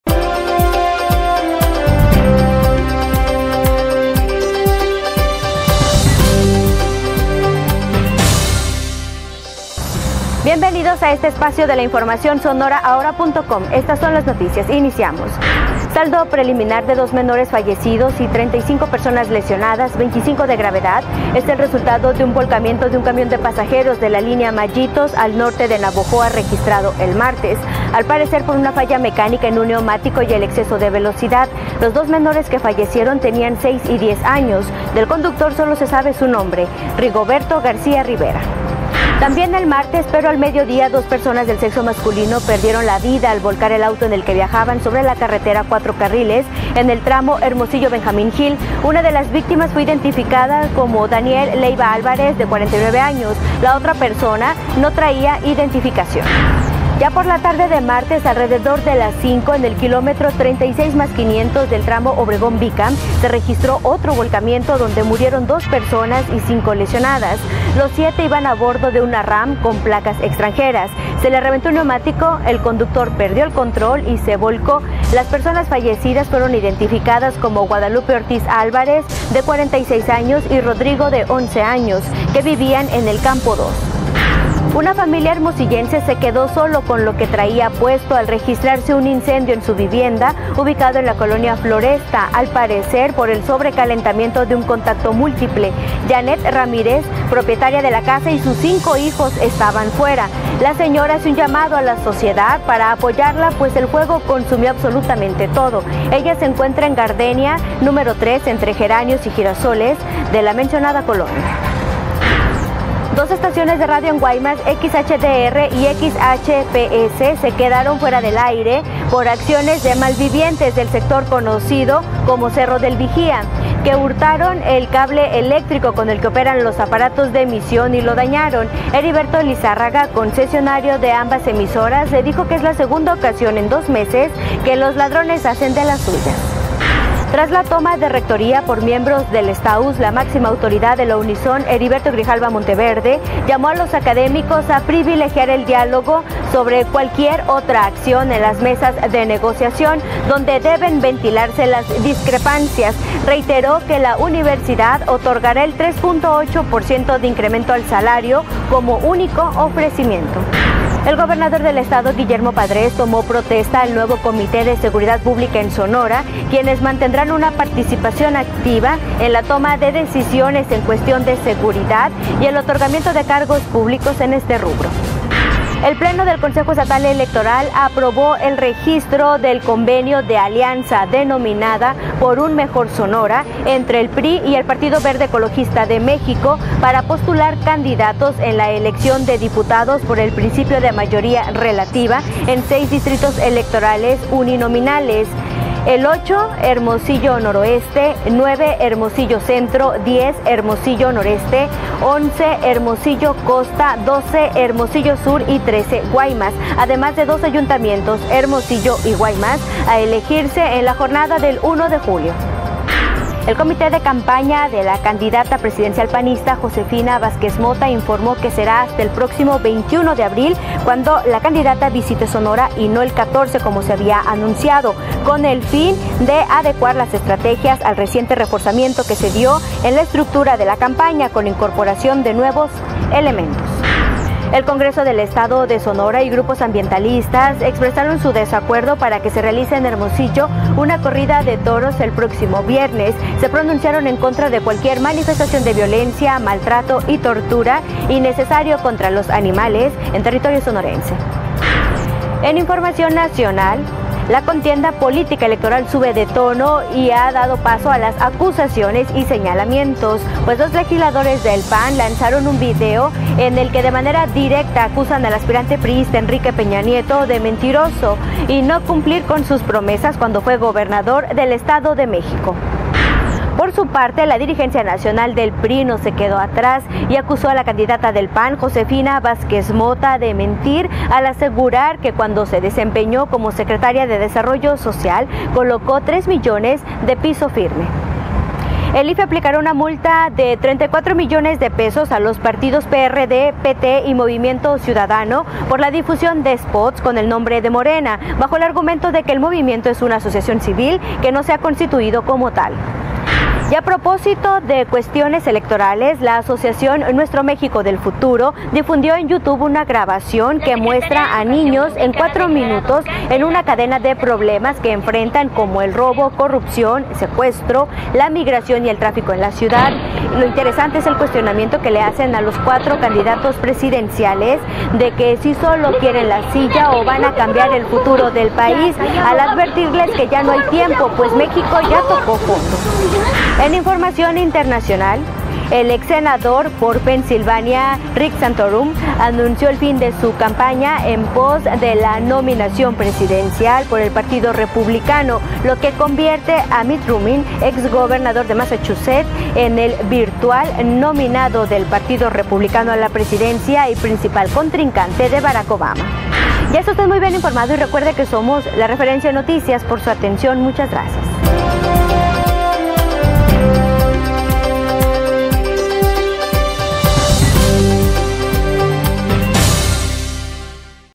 El noticiario sonoraahoratv 11042012.mp4
VUELCA MAYITOS: MUEREN 2 MENORES
Saldo preliminar de 2 menores fallecidos y 35 personas lesionadas 25 de gravedad, es el resultado un volcamiento de un camión de pasajeros de la línea mayitos al Norte de Navojoa registrado el martes, al parecer  por una falla mecánica en un neumático y el exceso de velocidad. Los dos menores que fallecieron tenían 6 y 10 años. Del conductor solo se sabe su nombre, Rigoberto García Rivera.

FALLECEN EN ACCIDENTE
También el martes pero al medio día, dos personas del sexo masculino perdieron la vida al volcar el auto en el que viajaban sobre la Carretera Cuatro Carriles en el tramo Hermosillo-Benjamín Hill. Una de las víctimas fue identificada como Daniel Leyva Alvarez, de 49 años y la otra persona no portaba identificación.

OTRO VOLCAMIENTO
Ya por la tarde de martes alrededor de las 5, en el kilómetro 36 + 500 del tramo Obregón-Vícam se registro otro volcamiento donde murieron dos personas y cinco resultaron lesionadas. Los siete iban a bordo de una RAM con placas extranjeras,Las personas fallecidas fueron identificadas como Guadalupe Ortiz Álvarez de 46 años y Rodrigo de 11 años, ambos vivían en el Campo 2.

INCENDIO ACABA CON VIVIENDA
Una familia hermosillense se quedo solo con lo que traían puesto al registrarse un incendio en su vivienda ubicada en la colonia Floresta al parecer por el sobrecalentamiento de un contacto múltiple. Janeth Ramírez, propietaria de la casa y sus 5 hijos estaban fuera

LE PEGAN VANDALOS A RADIODIFUSORAS
Dos estaciones de radio en Guaymas XHDR y XHPS se quedaron fuera del aire  por acciones de malvivientes del sector conocido como Cerro del Vigía,

RECTOR LLAMA AL DIALOGO
Tras la toma de rectoría por miembros del STAUS, la máxima autoridad de la UNISON Heriberto Grijalva Monteverde llamo a los académicos a privilegiar el diálogo sobre cualquier otra acción .

FORMAN COMITÉ DE SEGURIDAD PÚBLICA
El gobernador del Estado, Guillermo Padrés tomó protesta al nuevo Comité de Seguridad Pública en Sonora quienes mantendrán una participación activa en la toma de decisiones en cuestión de seguridad y el otorgamiento de cargos públicos en este rubro.

AVALAN ALIANZA
El pleno del Consejo Estatal Electoral  aprobó el registro del convenio de alianza denominada "Por un mejor Sonora" entre el PRI y el Partido Verde Ecologista de México, para postular candidatos en la elección de diputados por el principio de mayoría relativa en seis distritos electorales uninominales que son el VIII Hermosillo Noroeste, IX Hermosillo Centro, X Hermosillo Noreste, XI Hermosillo Costa, XII Hermosillo Sur y XIII Guaymas, además  de dos ayuntamientos: Hermosillo y Guaymas, a elegirse en la jornada electoral del 1 de Julio.

SE MANIFIESTAN ANTITAURINOS
El Congreso del Estado de Sonora y grupos ambientalistas expresaron su desacuerdo para que se realice en Hermosillo una corrida de toros el próximo viernes y se pronunciaron en contra de cualquier manifestación de violencia, maltrato, tortura y sufrimiento innecesarios contra los animales en territorio sonorense.

PAN ACUSA DE MENTIROSO A PEÑA NIETO
La contienda política-electoral sube de tono y ha dado paso a las acusaciones y señalamientos pues los legisladores del (PAN) lanzaron un nuevo video en el que de manera directa acusan al aspirante priísta Enrique Peña Nieto de mentiroso y de no cumplir con sus promesas cuando fue gobernador del Estado de México.

VAZQUEZ MOTA MIENTE
Por su parte la dirigencia nacional del (PRI) no se quedo atrás y acusó a la candidata del (PAN) Josefina Vázquez Mota, de mentir al asegurar que cuando se desempeñó como secretaria de Desarrollo Social colocó tres millones de "Pisos Firmes", mensajes que son falacias señalaron.

IFE MULTARA A IZQUIERDISTAS
El (IFE) aplicará una multa global de casi 34 millones de pesos a los partidos (PRD),  (PT) y  Movimiento Ciudadano, por la difusión de spots con el nombre de Morena bajo el argumento de que el Movimiento es una asociación civil que no se ha constituido como tal.

NIÑOS INCOMODOS
Y a propósito de la cuestión electoral, la asociación Nuestro México del Futuro difundió en YouTube una grabación que muestra a niños en cuatro minutos en una cadena de problemas que enfrentan diariamente como robo, corrupción, secuestro, contaminación, la migración y el tráfico en la ciudad, lo interesante es el cuestionamiento que le hacen a los cuatro candidatos presidenciales de que si sólo quieren "la silla" o van a cambiar el futuro del país, al advertirles que ya no hay tiempo, pues "México ya tocó fondo".

SE VA SANTORUM
El ex senador por Pennsylvania, Rick Santorum, anuncio  el fin de su campaña en pos de la nominación presidencial por el partido republicano, lo que convierte a Mitt Romney,  ex gobernador de Massachusetts, en el virtual nominado del partido republicano a la presidencia y principal contrincante de Barack Obama.